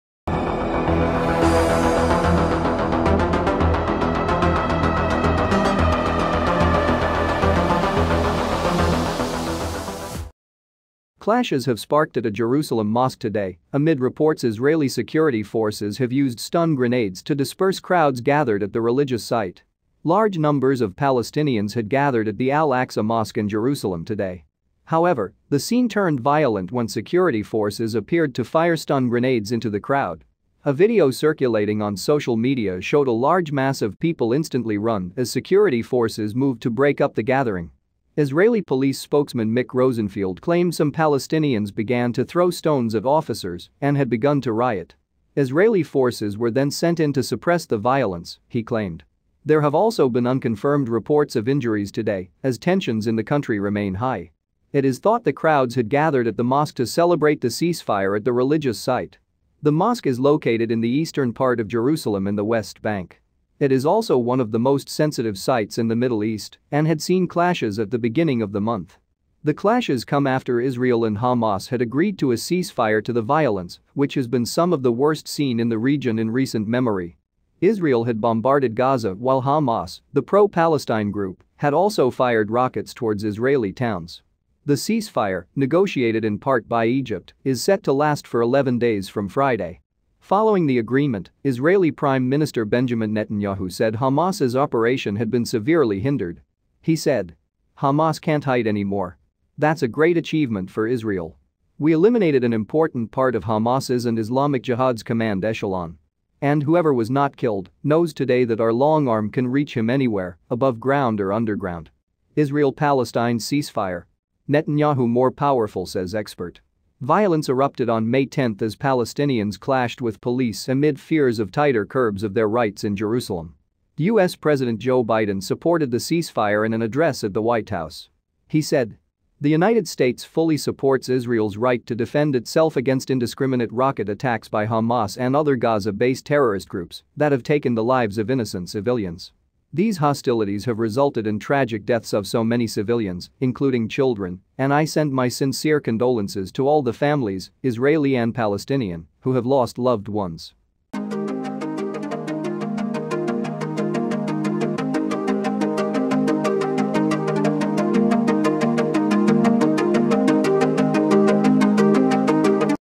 Israel ceasefire on brink as Jerusalem mosque clashes erupt – police fire stun grenades
CLASHES have sparked at a Jerusalem mosque today amid reports Israeli security forces have used stun grenades to disperse crowds gathered at the religious site. Large numbers of Palestinians had gathered at the Al-Aqsa Mosque in Jerusalem today. However, the scene turned violent when security forces appeared to fire stun grenades into the crowd. A video circulating on social media showed a large mass of people instantly run as security forces moved to break up the gathering. Israeli police spokesman Mick Rosenfield claimed some Palestinians began to throw stones at officers and had begun to riot. Israeli forces were then sent in to suppress the violence, he claimed.  There have also been unconfirmed reports of injuries today as tensions in the country remain high. It is thought the crowds had gathered at the mosque to celebrate the ceasefire at the religious site. The mosque is located in the eastern part of Jerusalem in the West Bank. It is also one of the most sensitive sites in the Middle East and had seen clashes at the beginning of the month. The clashes come after Israel and Hamas had agreed to a ceasefire to the violence which has been some of the worst seen in the region in recent memory. Israel had bombarded Gaza while Hamas, the pro-Palestine group, had also fired rockets towards Israeli towns. The ceasefire, negotiated in part by Egypt is set to last for 11 days from Friday.  Following the agreement, Israeli Prime Minister Benjamin Netanyahu said Hamas' operation had been severely hindered. He said: "Hamas can't hide anymore. "That's a great achievement for Israel. "We eliminated an important part of Hamas's and Islamic Jihad's command echelon. "And whoever was not killed, knows today that our long arm can reach him anywhere, above ground or underground." Israel-Palestine ceasefire: Netanyahu 'more powerful' says expert. Violence erupted on May 10 as Palestinians clashed with police amid fears of tighter curbs of their rights in Jerusalem. US President Joe Biden supported the ceasefire in an address at the White House. He said: "The United States fully supports Israel’s right to defend itself against indiscriminate rocket attacks by Hamas and other Gaza-based terrorist groups that have taken the lives of innocent civilians. "These hostilities have resulted in tragic deaths of so many civilians, including children, and I send my sincere condolences to all the families, Israeli and Palestinian, who have lost loved ones."